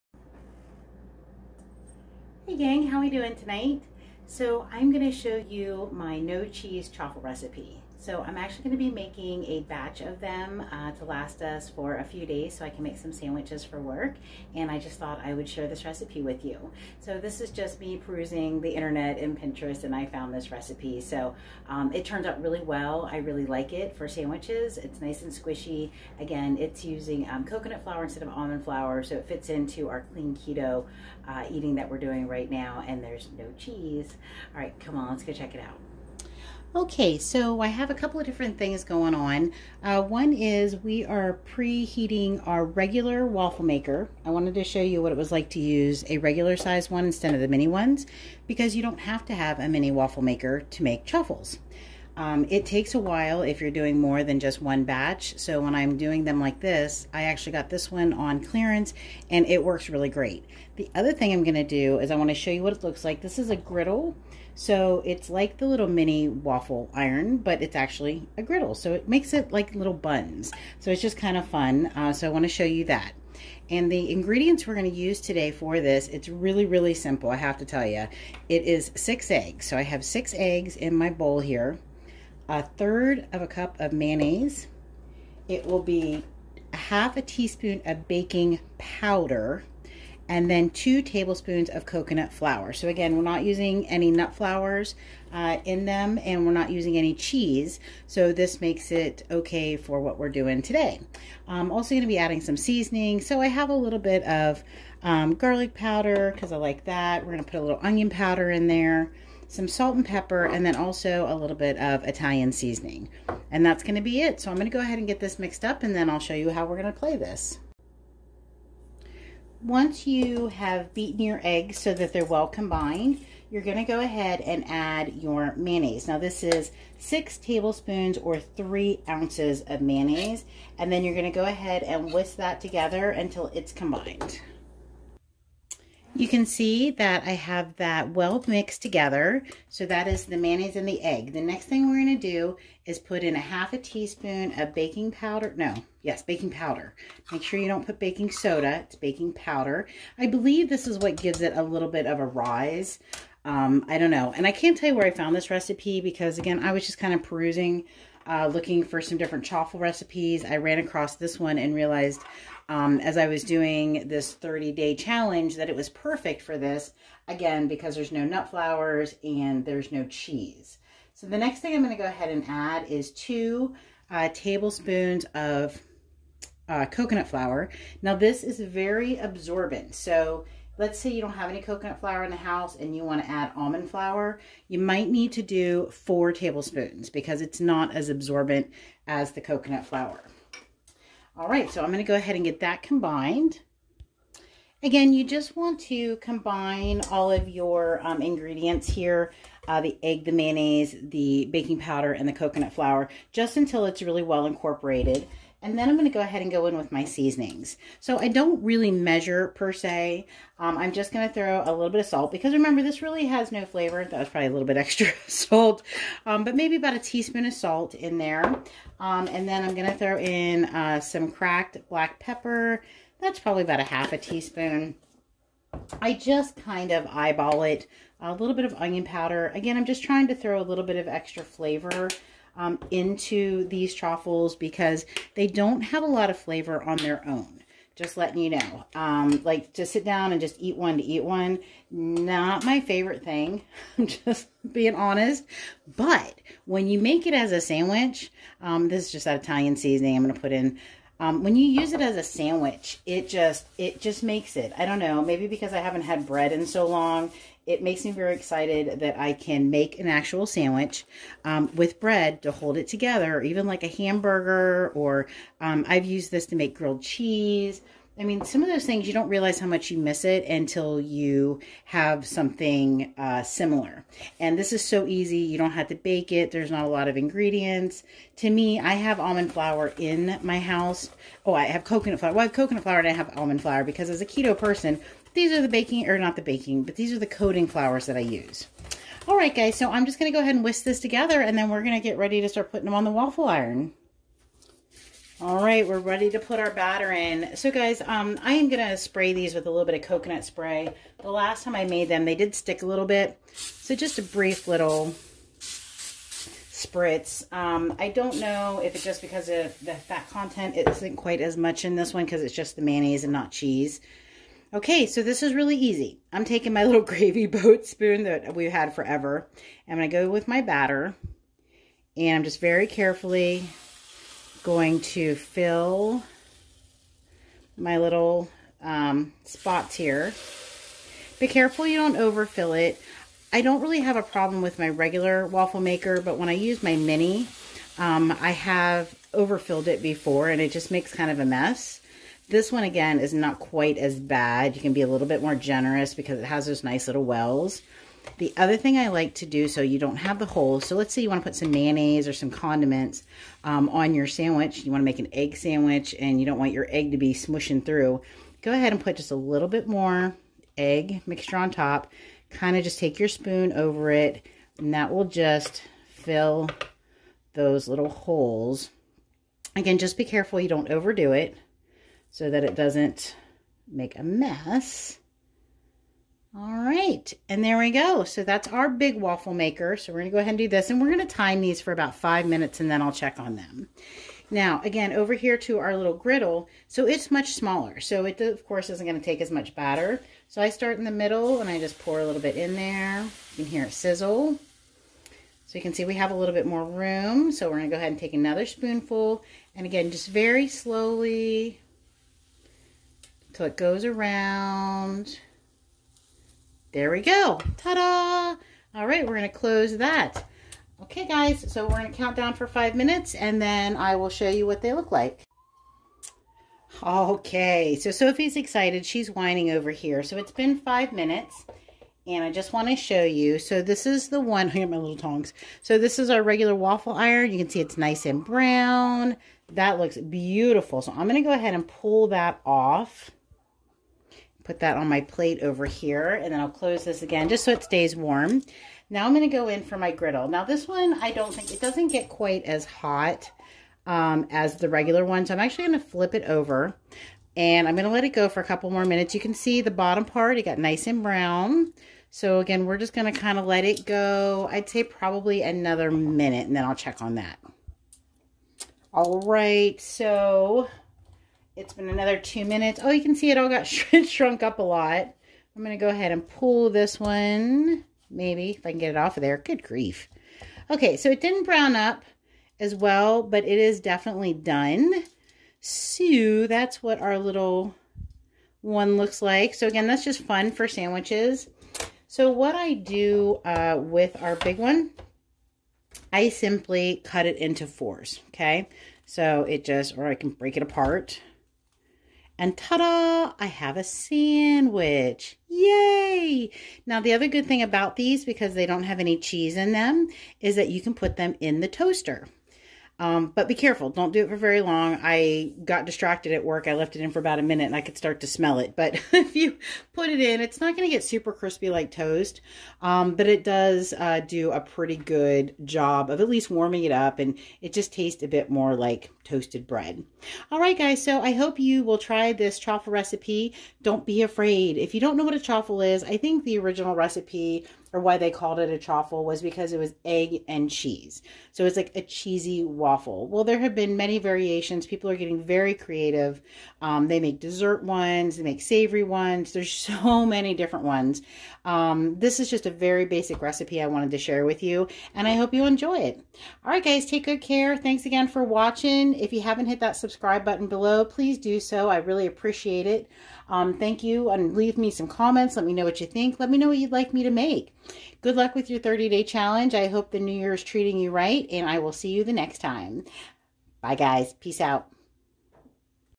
No Cheese Chaffles! Great for Clean Keto!
Today I was making a batch of chaffles to use as bread for sandwiches and I wanted to share this recipe with you! It is perfect for our 30 day clean keto challenge. They are really good if you pop it in the toaster for about 30-45 seconds to warm them up. Let me know what you think! This made 3 big chaffles and 2 in the griddle. (12 small chaffles)
Ingredients:
6 eggs, beaten
6 Tablespoon or 3 oz Mayo (1/3 C)
1/2 teaspoon Baking powder
2 Tablespoon Coconut flour
Seasonings of your choice!

If you want to try a smaller portion (This should make you 2 small chaffles)
1 egg, beaten
1 teaspoon coconut flour
1 Tablespoon mayo